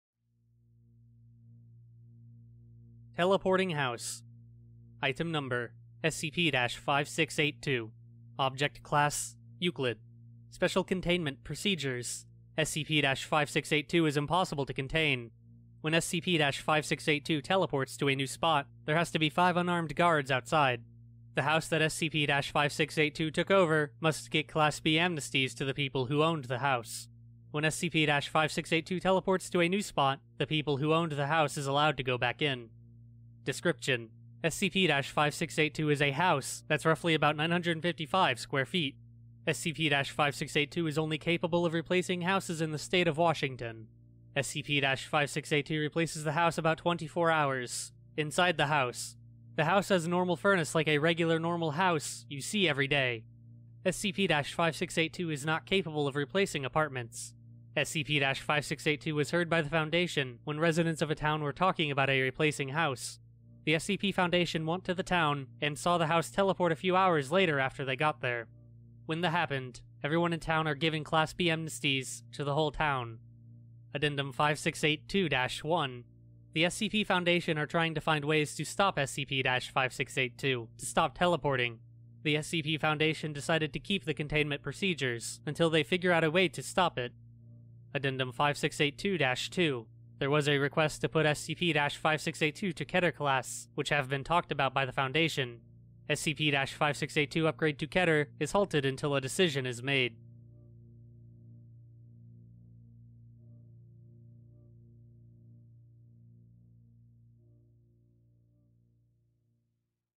SCP-5682 "Teleporting House" [Community SCP]
Little bigger than a phone booth.

Article by "freddy and foxy".